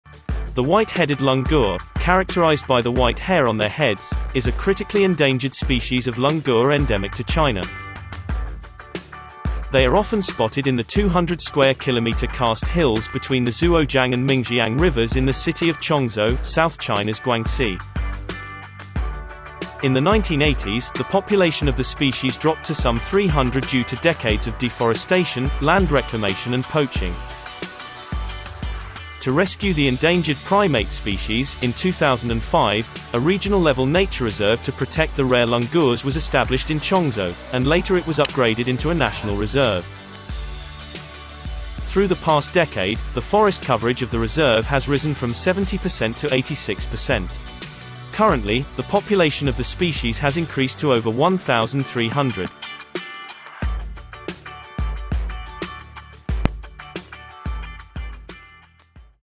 GLOBALink | White-headed langurs' population improves thanks to habitat protection
The white-headed langur is a critically endangered species of langur endemic to China. Through years of animal protection efforts, the population of the species has increased from about 300 to over 1,300.